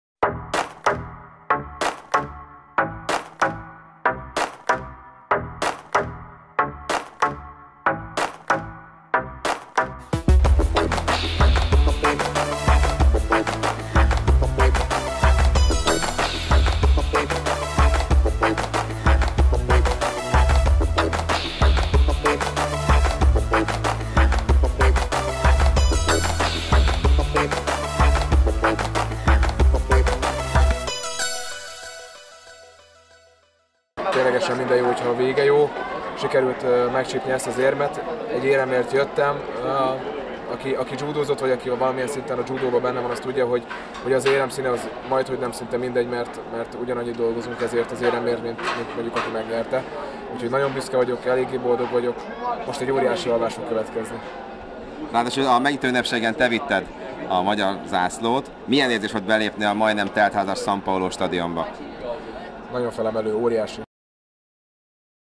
Ötvenöt ország között a tizennegyedik helyet szerezték meg judósaink a nápolyi Universiadén!🥋🎓
A mieink egy bronzéremmel, egy ötödik és három hetedik helyezéssel zárták a rangos eseményt. 🤩🥳

Nézzétek meg a bronzérmes Tóth Krisztiánnal (Testnevelési Egyetem (TF)) készített inetrjúnkat a videó végén. 🤓🥋